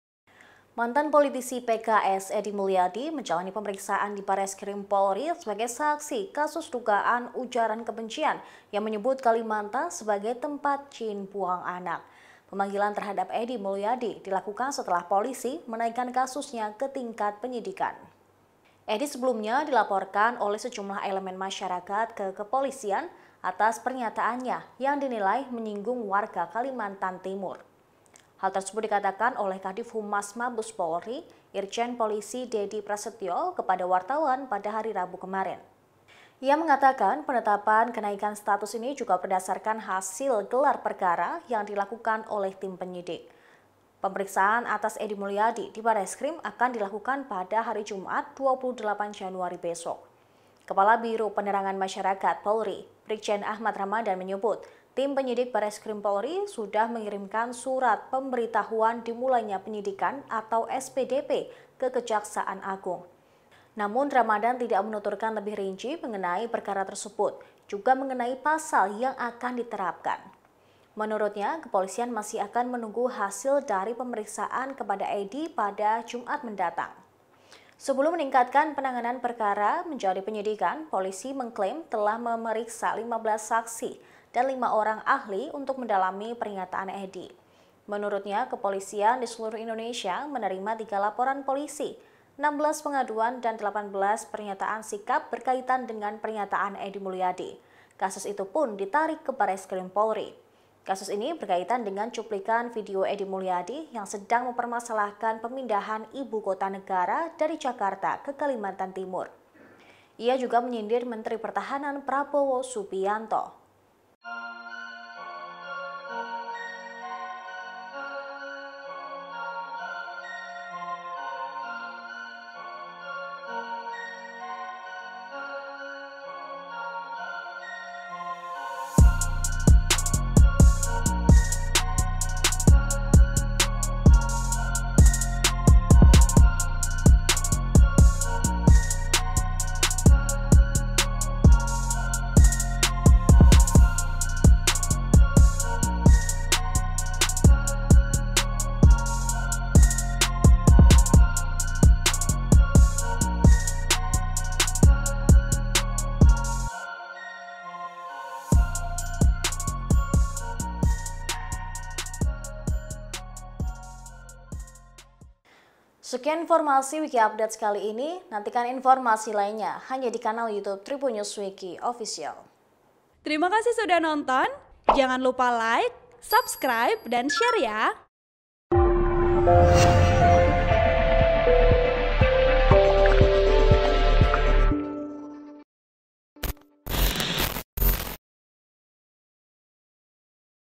Besok Edy Mulyadi akan Diperiksa Bareskrim Polri, Status Perkara Dinaikkan ke Penyidikan
TRIBUN-VIDEO.COM - Eks politisi PKS  Edy Mulyadi menjalani pemeriksaan di Bareskrim Polri sebagai saksi kasus dugaan ujaran kebencian yang menyebut Kalimantan sebagai tempat jin buang anak.

Edy sebelumnya dilaporkan oleh sejumlah elemen masyarakat ke kepolisian atas pernyataannya yang dinilai menyinggung warga Kalimantan Timur (Kaltim).

Hal tersebut dikatakan oleh Kadiv Humas Mabes Polri Irjen Pol Dedi Prasetyo kepada wartawan, Rabu (26/1/2022).

"Disimpulkan bahwa Perkara Ujaran Kebencian oleh Edy telah ditingkatkan statusnya dari tahap penyelidikan ke tahap penyidikan," kata Irjen Pol Dedi Prasetyo.

Ia mengatakan penetapan kenaikan status ini juga berdasar pada hasil gelar perkara yang dilakukan tim penyidik.

Pemeriksaan atas Edy Mulyadi di Bareskrim akan dilakukan pada Jumat (28/1) besok.

Kepala Biro Penerangan Masyarakat (Karopenmas) Polri Brigjen Ahmad Ramadhan menyebut, Tim penyidik Bareskrim Polri sudah mengirimkan Surat Pemberintahuan Dimulainya Penyidikan (SPDP) ke Kejaksaan Agung.

Kepala Biro Penerangan Masyarakat (Karopenmas) Polri Brigjen Ahmad Ramadhan.

Namun Ramadhan tidak menuturkan lebih rinci mengenai perkara tersebut juga mengenai pasal yang akan diterapkan.

Menurutnya, kepolisian masih akan menunggu hasil dari pemeriksaan kepada Edy pada Jumat mendatang. "Nanti Jumat pasti updatenya bisa kami sampaikan," tambah dia.

Sebelum meningkatkan penanganan perkara menjadi penyidikan, polisi mengklaim telah memeriksa 15 saksi dan lima orang ahli untuk mendalami pernyataan Edy.

Menurutnya, kepolisian di seluruh Indonesia menerima tiga laporan polisi, 16 pengaduan dan 18 pernyataan sikap berkaitan dengan pernyataan Edy Mulyadi. Kasus itu pun ditarik ke Bareskrim.

Kasus itu berkaitan dengan cuplikan video Edy Mulyadi yang sedang mempermasalahkan pemindahan ibu kota negara dari Jakarta ke Kalimantan Timur.

Ia menyindir Menteri Pertahanan (Menhan) Prabowo Subianto.9ribun-video.com/tribunnews.com)

Artikel ini telah tayang di Tribunnews.com dengan judul Edy Mulyadi Hari Ini Jalani Pemeriksaan di Bareskrim, Status Perkara Naik ke Penyidikan,